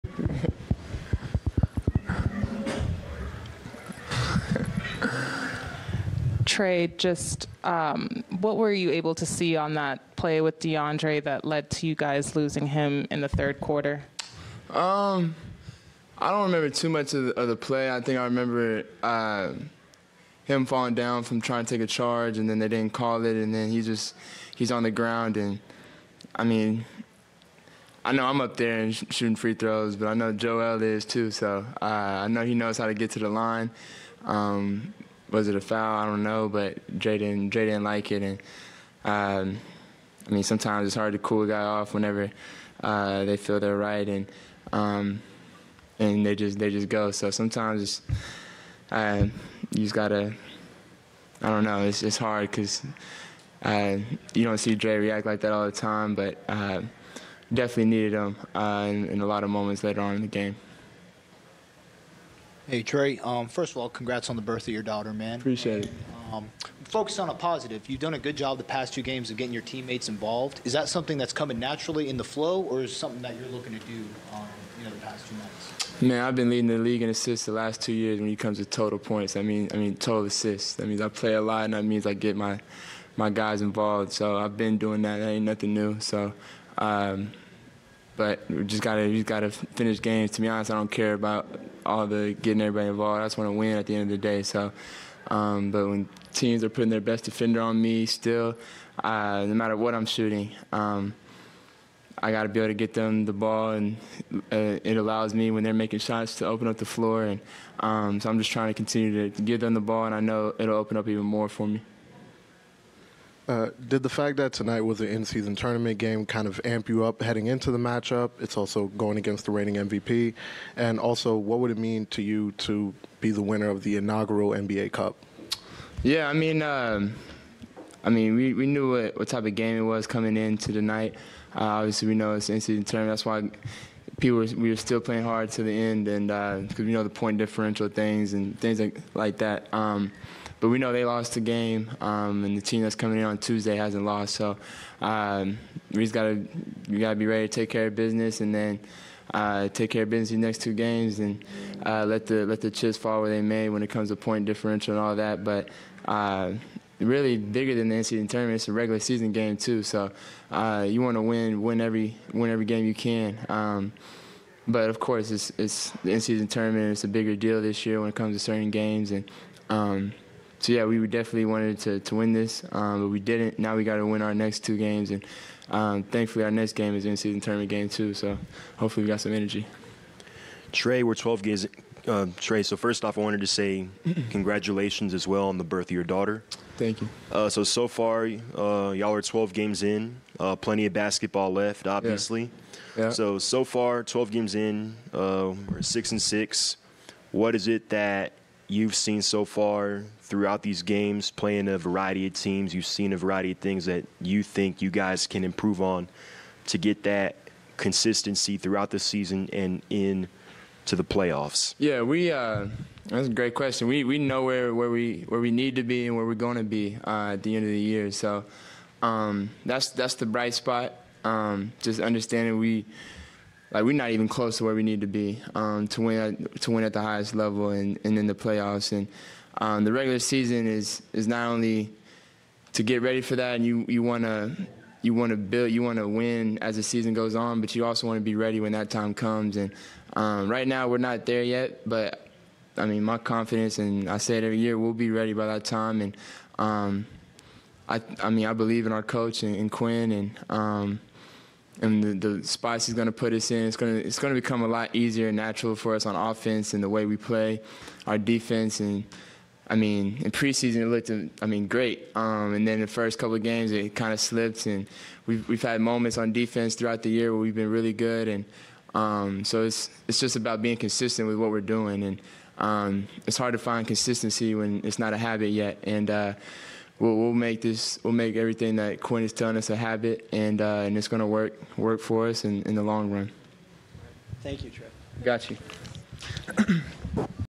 Hawks vs. 76ers Postgame Press Conference: Trae Young
Hawks vs. 76ers Postgame Press Conference: Trae Young after 126-116 loss.